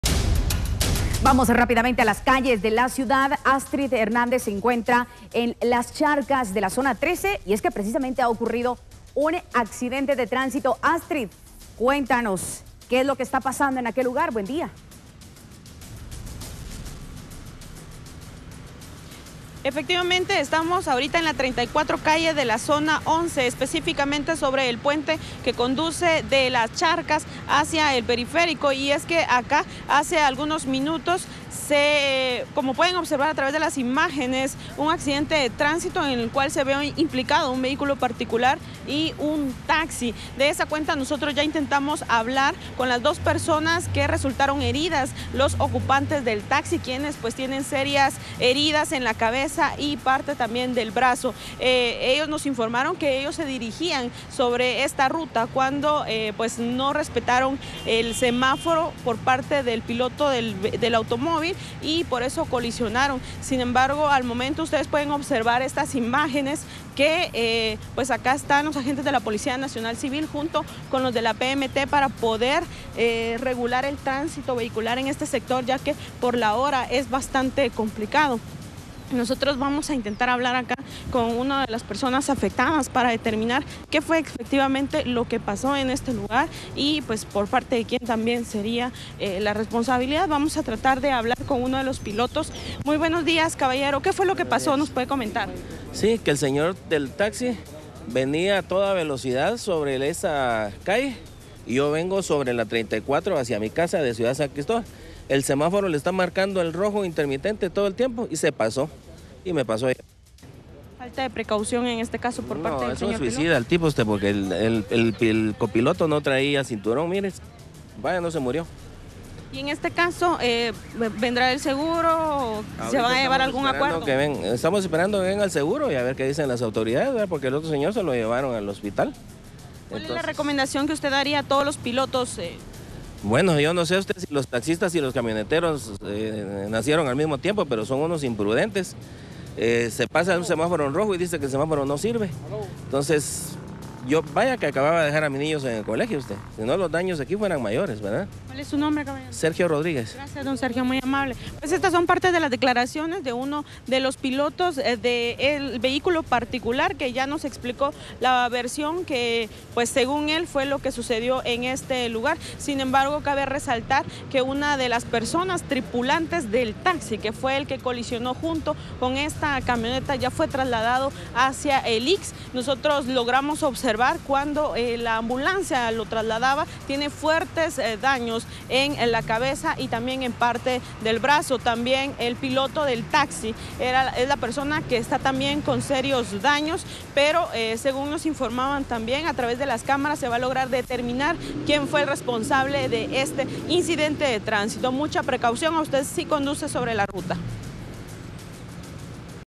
Accidente de tránsito en zona 11 deja dos personas heridas
En un sector de la zona 11 de la ciudad capital ocurrió un hecho de tránsito que involucró un vehículo particular y un taxi. Por el percance se registraron heridos y varios daños materiales. Autoridades policiales ya se encuentran en el lugar de los hechos regulando el tránsito vehicular